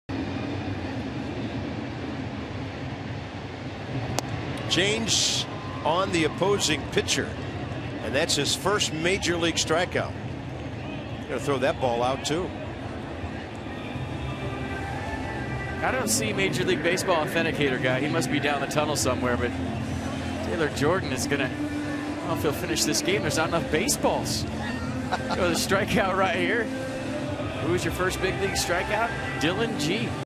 2013/06/29 Jordan's first career strikeout
WSH@NYM: Jordan fans Gee to collect first career K